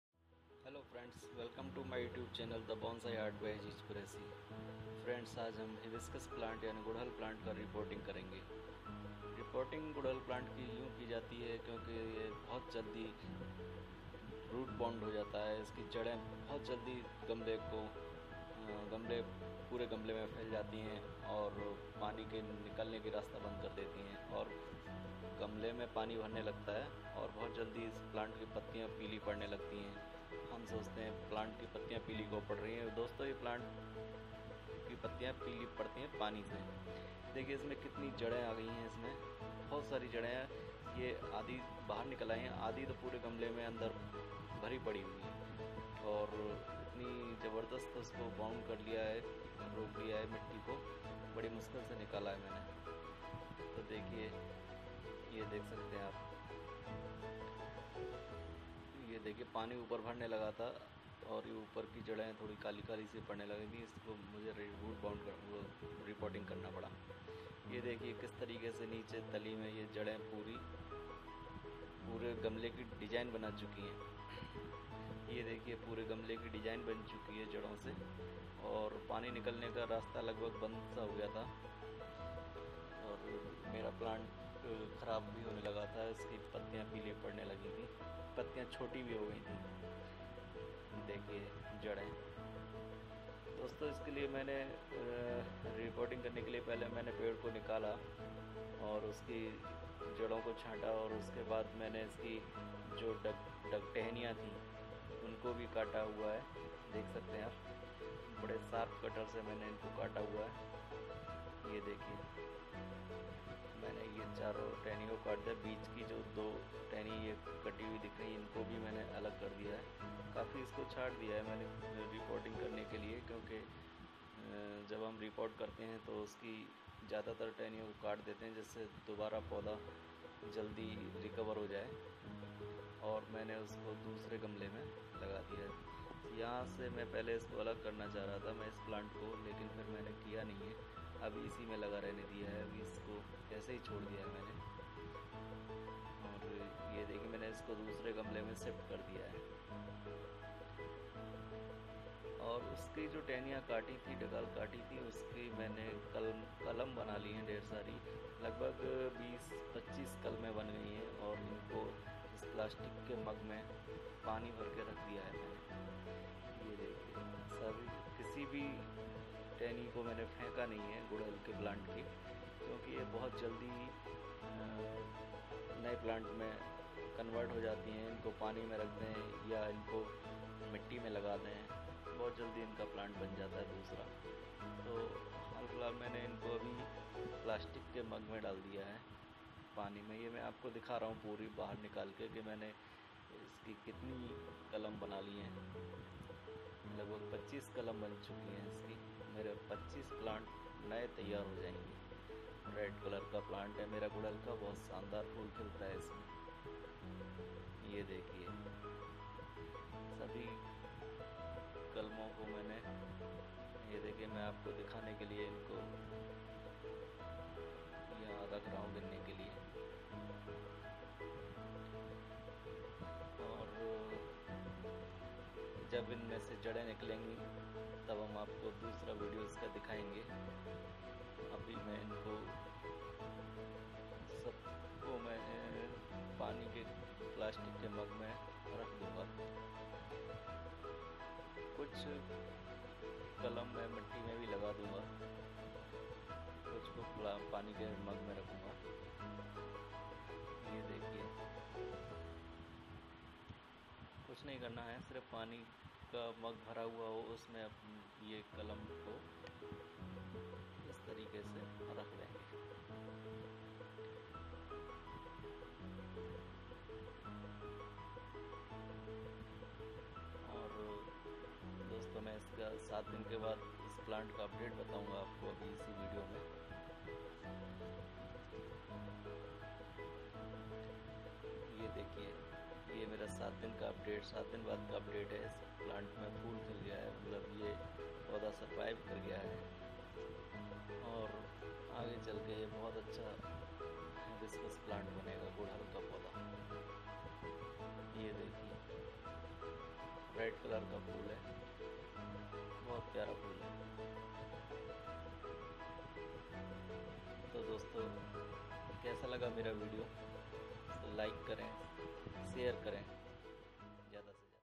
Hibiscus Plant Repotting with Update 05-July-2018
Freinds is video me aapko hibiscus plant ki Repotting karke dikhaunga. Pot me lagane ka sabse bada drawback yahi hai ke bahut hi jaldi rootbound ho jata h. To phir dekhiye aur enjoy kijiye.
Thanks